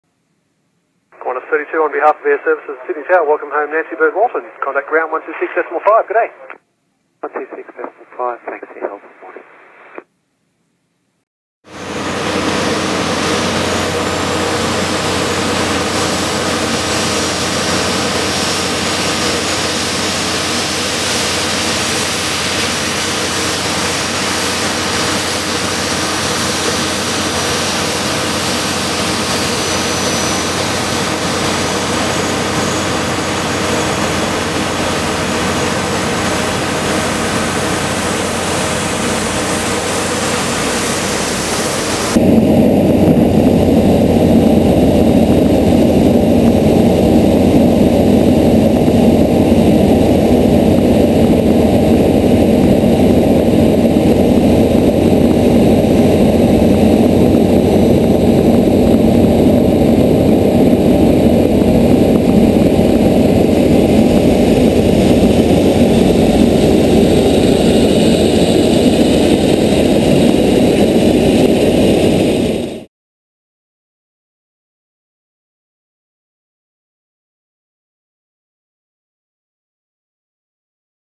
Airservices welcomes home Qantas A380 Nancy-Bird Walton
Airservices air traffic controllers and aviation fire fighters welcomed the return of Qantas' flagship A380 VH-OQA,  Nancy-Bird Walton, to Sydney on 22 April.

Call-signed 'QF32' (Qantas flight 32), the A380 touched down on Sydney's runway 16R at about 9:20am, having flown in from Singapore.

The occasion was marked with a special welcome radio transmission by the tower controller.

"Qantas 32, on behalf of Airservices at Sydney tower, welcome home Nancy-Bird Walton." the controller said.

On landing, the aircraft was also treated to a traditional 'water arch salute' by Airservices aviation fire fighters.

The water arch salute involved two ultra-large Mk8 fire vehicles throwing water over a distance of more than 80 metres as the aircraft passed through the arch.

The Nancy-Bird Walton was grounded on 4 November 2010 when an uncontained engine failure struck shortly after take-off from Singapore, resulting in extensive damage.